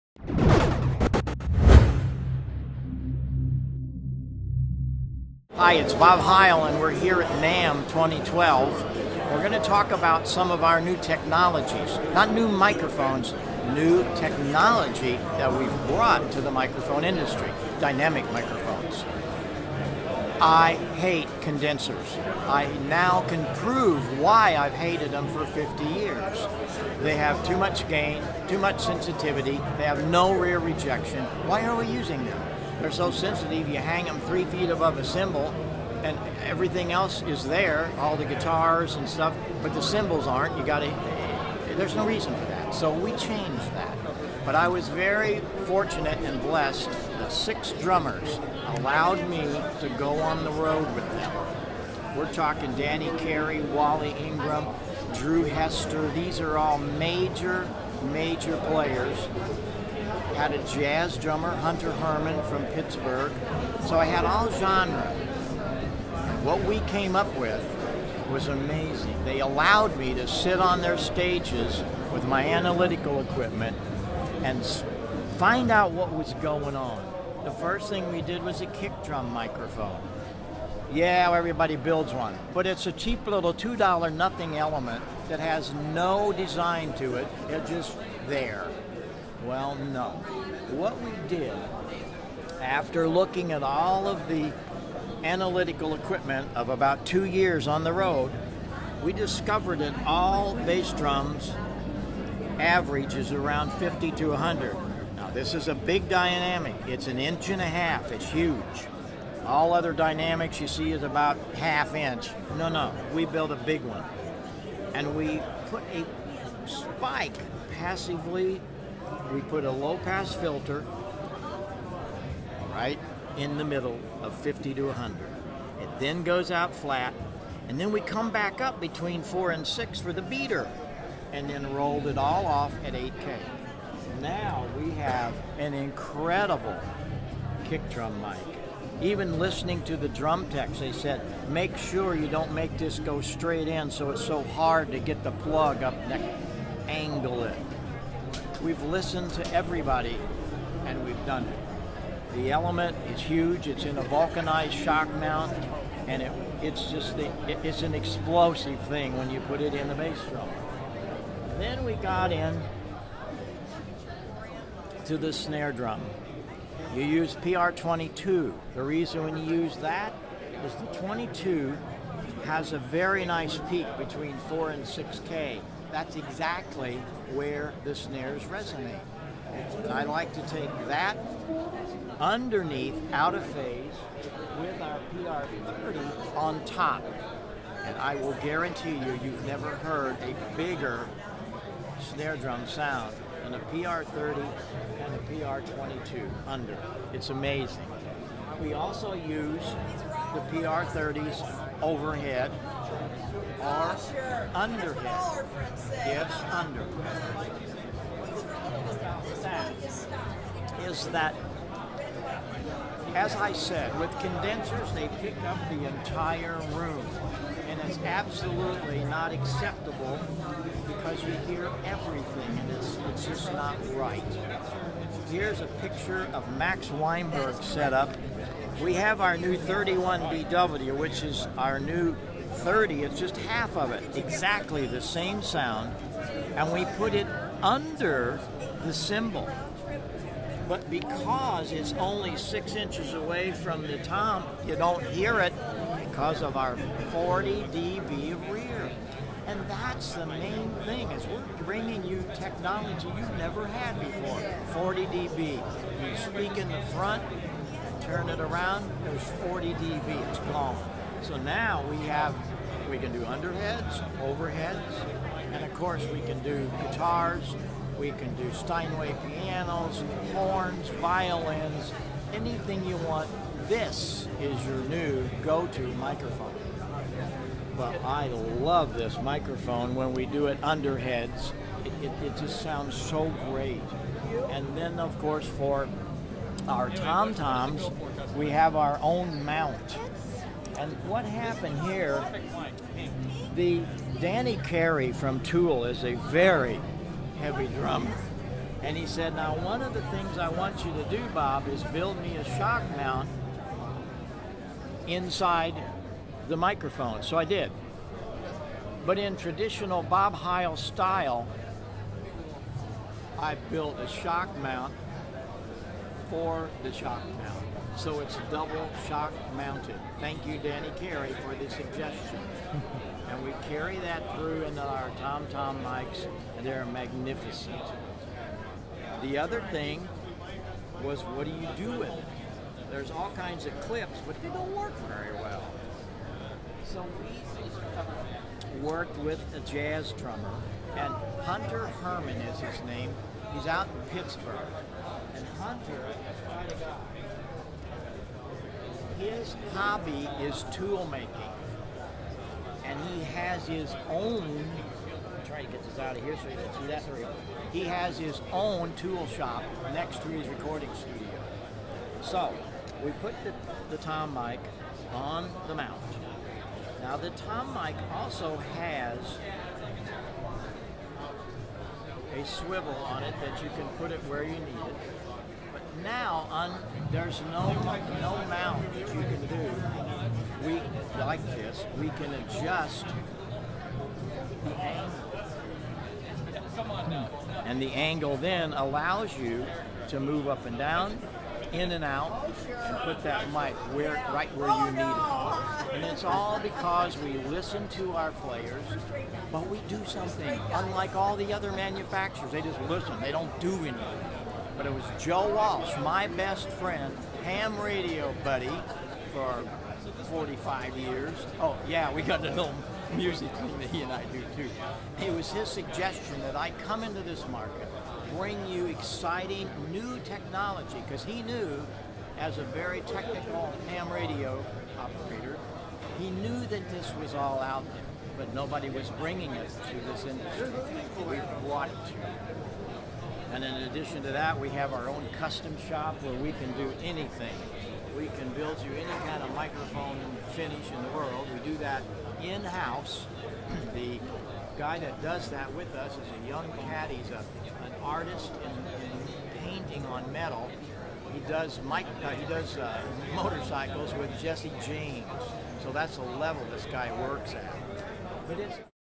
Heil Sound At NAMM 2012
Get Your Free DRUM! Mag Pack At the 2012 NAMM Show, we got the chance to chat with Bob Heil, the legendary sound engineer who toured with The Grateful Dead and The Who and single-handedly revolutionized the technology used for modern rock sound systems. Heil excitedly showed us the new Heil Sound PR drum microphones, which he developed with six top drummers spanning a variety of styles. Judging by his description, he put every aspect of drum microphone design under the microscope.